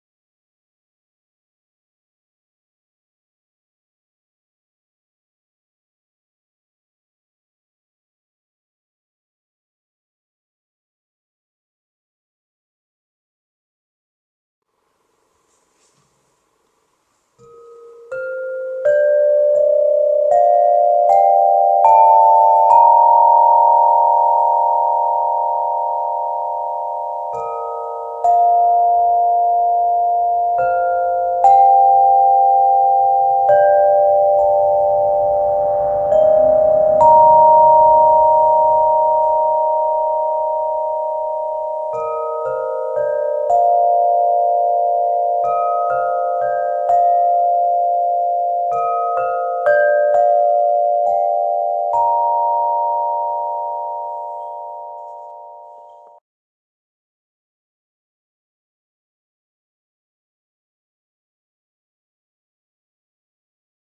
Kristallen Harp Magna Mater
Kristallen Harp Luistervoorbeeld van Magna Mater, leverancier van kwalitatief hoogwaardige klankinstrumenten zoals roestvrijstalen stemvorken Zie
Helaas zijn we moeten stoppen met de verkoop van de kristallen harpen en stemvorken i.v.m. teveel breuk, vooral tijdens het vervoer.